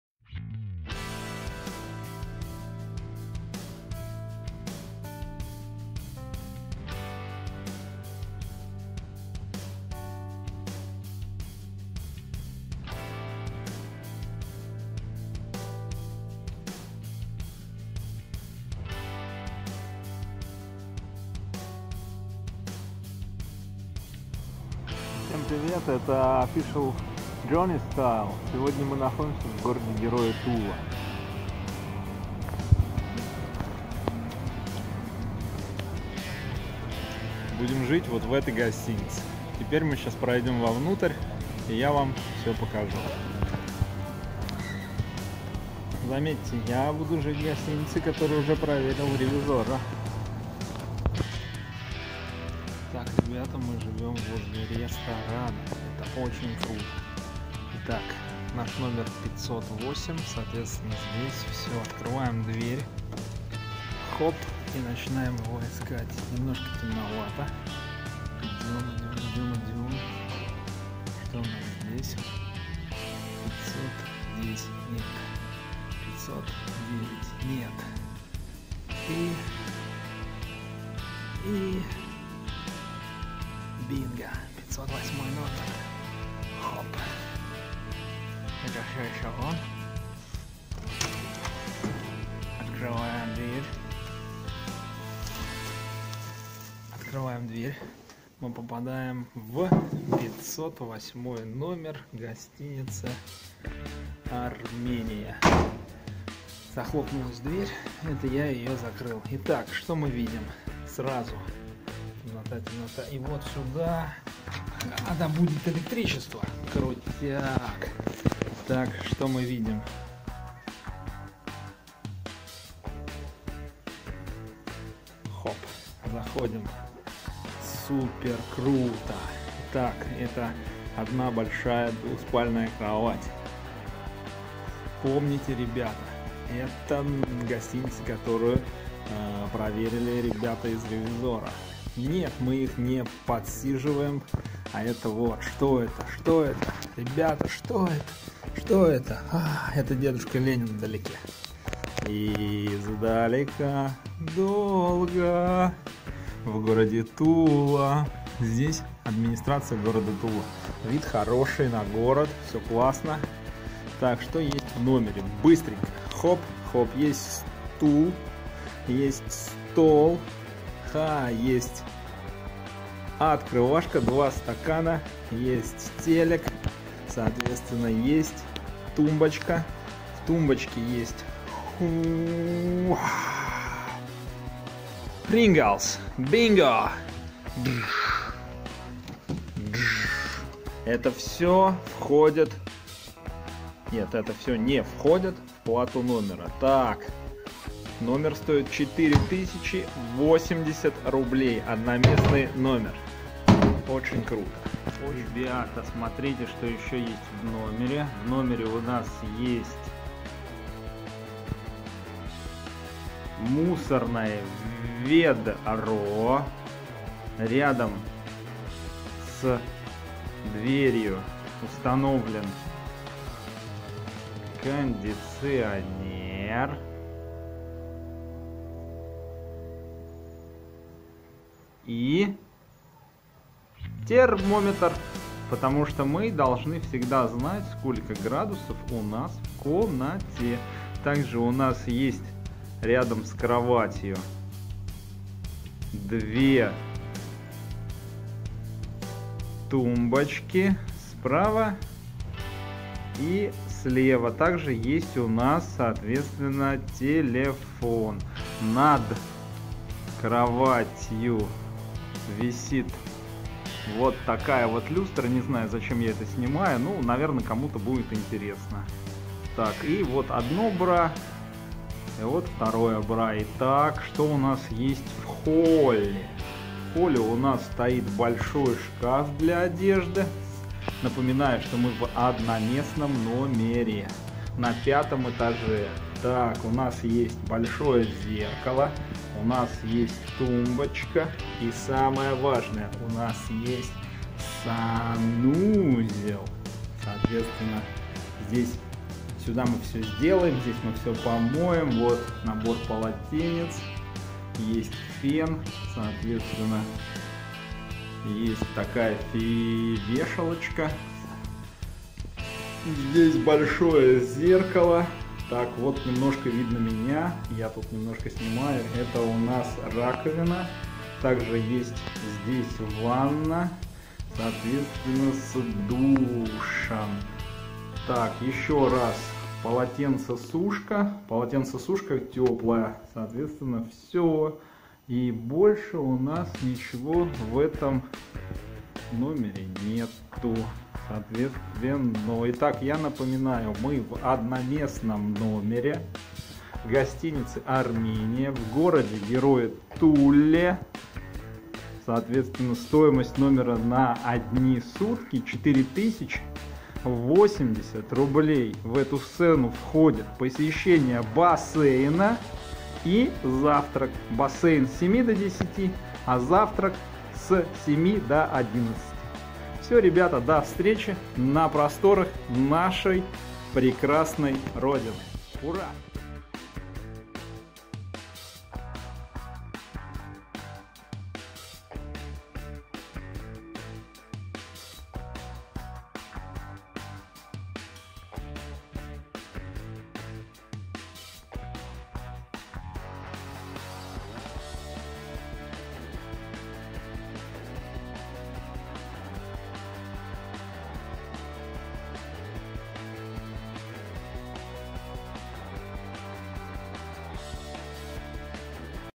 4080 рублей за ночь в Тульском Ереване. Отличное место на пару ночей. Гостиницы и отели.
Быстрый взгляд на одноместный номер в Тульской гостинице Армения. По стопам ревизорро. Хорошее место для хорошего человека.